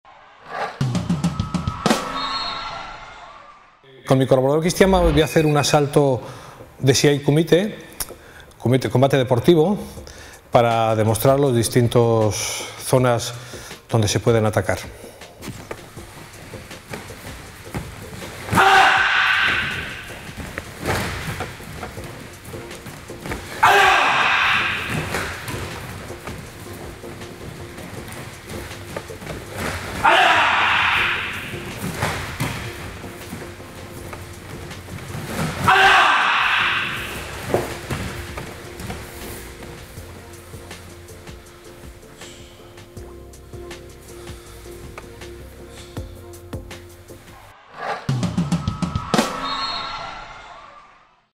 Karate 10. Asalto de siai kumite (combate deportivo)
José Silva, cinturón negro 5º Dan e instructor nacional de karate, nos hace una demostración de un asalto de Siai Kumite o combate deportivo.

Agradecimiento: Club Deportivo Artes Marciales Valladolid